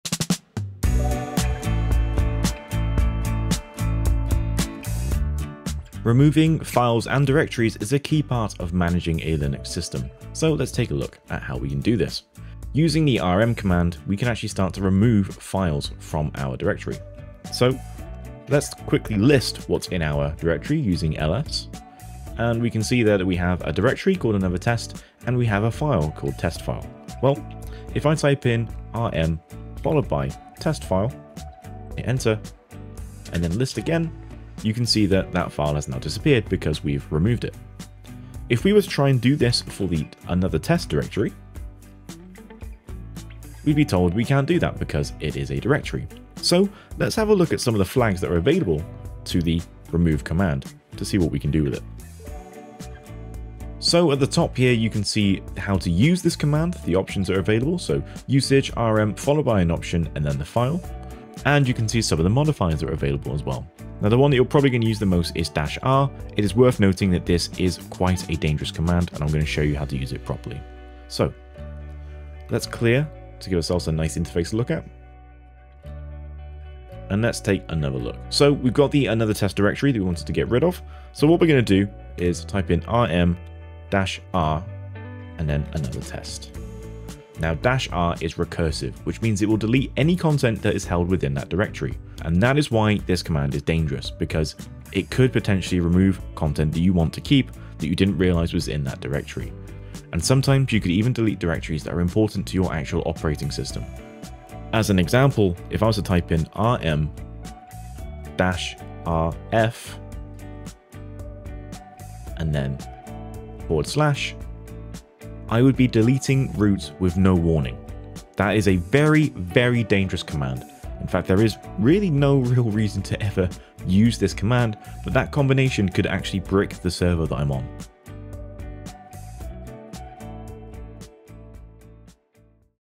Removing Files and Directories - Linux, Quickly
In this video I will quickly show you how to use the rm command to remove files, and then how to use modifiers to perform other actions such as removing directories with rm -r.